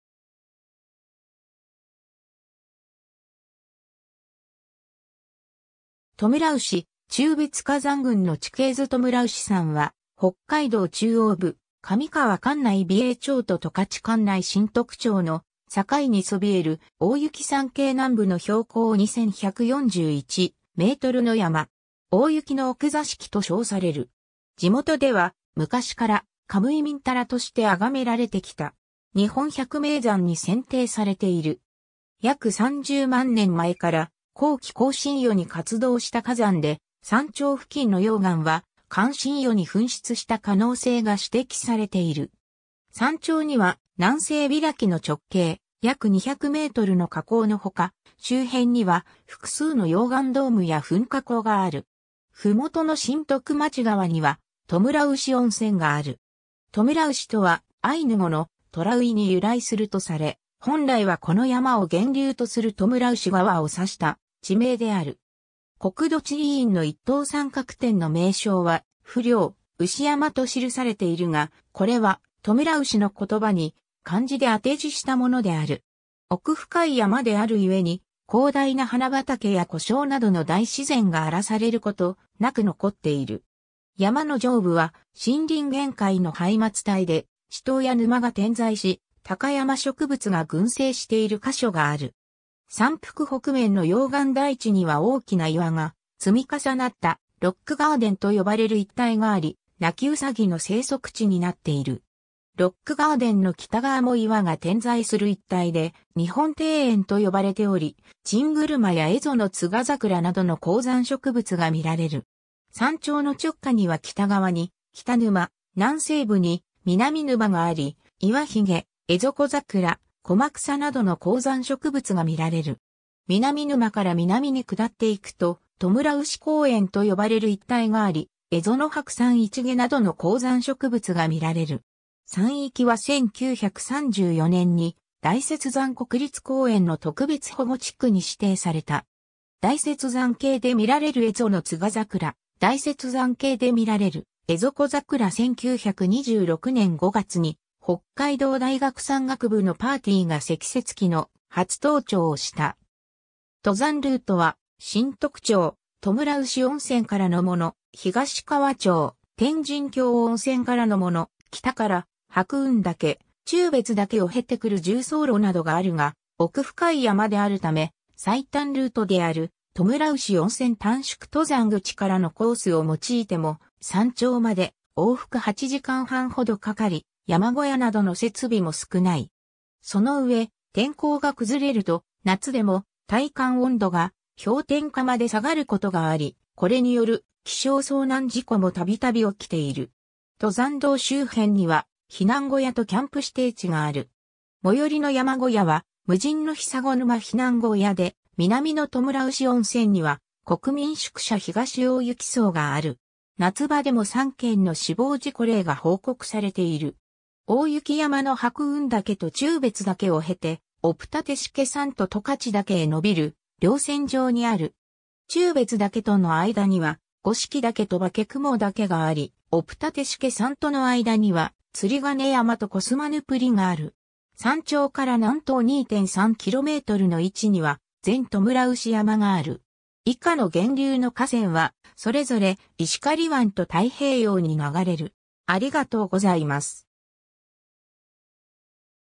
トムラウシ山
トムラウシ・忠別火山群の地形図 トムラウシ山（富村牛山、富良牛山）は、北海道中央部、上川管内美瑛町と十勝管内新得町の境にそびえる大雪山系南部の標高2,141 mの山。
「大雪の奥座敷」と称される。
地元では昔から「カムイミンタラ」（神々の遊ぶ庭）として崇められてきた。
日本百名山に選定されている。
 約30万年前から後期更新世に活動した火山で、山頂付近の溶岩は完新世に噴出した可能性が指摘されている。
山頂には南西開きの直径約200mの火口のほか、周辺には複数の溶岩ドームや噴火口がある。
麓の新得町側にはトムラウシ温泉がある。
 「トムラウシ」とは、アイヌ語の「トㇺラ・ウㇱ・イ」（緑色の藻の一種が群生するところ）に由来するとされ、本来はこの山を源流とするトムラウシ川を指した地名である。
国土地理院の一等三角点の名称は「富良牛山」と記されているが、これは「トムラウシ」の言葉に漢字で当て字したものである。
 奥深い山である故に、広大な花畑や湖沼などの大自然が荒らされることなく残っている。
山の上部は森林限界のハイマツ帯で、池塘や沼が点在し高山植物が群生している箇所がある。
山腹北面の溶岩台地には大きな岩が積み重なった「ロックガーデン」と呼ばれる一帯があり、ナキウサギの生息地になっている。
ロックガーデンの北側も岩が点在する一帯で「日本庭園」と呼ばれており、チングルマやエゾノツガザクラなどの高山植物が見られる。
山頂の直下には北側に北沼、南西部に南沼があり、イワヒゲ、エゾコザクラ、コマクサなどの高山植物が見られる。
南沼から南に下っていくと「トムラウシ公園」と呼ばれる一帯があり、エゾノハクサンイチゲなどの高山植物が見られる。
山域は1934年（昭和5年）に大雪山国立公園の特別保護地区に指定された。
 大雪山系で見られるエゾノツガザクラ 大雪山系で見られるエゾコザクラ 1926年（大正15年）5月に、北海道大学山岳部のパーティーが積雪期の初登頂をした。
 登山ルートは、新得町・トムラウシ温泉からのもの、東川町・天人峡温泉からのもの、北から白雲岳・忠別岳を経てくる縦走路などがあるが、奥深い山であるため、最短ルートであるトムラウシ温泉短縮登山口からのコースを用いても山頂まで往復8時間半ほどかかり、山小屋などの設備も少ない。
その上、天候が崩れると夏でも体感温度が氷点下まで下がることがあり、これによる気象遭難事故もたびたび起きている。
 登山道周辺には、避難小屋とキャンプ指定地がある。
最寄りの山小屋は、無人のヒサゴ沼避難小屋で、南のトムラウシ温泉には国民宿舎東大雪荘がある。
 夏場でも3件の死亡事故例が報告されている。
 大雪山の白雲岳と忠別岳を経て、オプタテシケ山と十勝岳へ延びる稜線上にある。
忠別岳との間には、五色岳 (1,868 m) と化雲岳 (1,954 m) があり、オプタテシケ山との間には、ツリガネ山 (1,708 m) とコスマヌプリ (1,626 m) がある。
山頂から南東2.3 kmの位置には、前トムラウシ山 (1,649 m) がある。
 以下の 源流の河川は、それぞれ石狩湾と太平洋に流れる。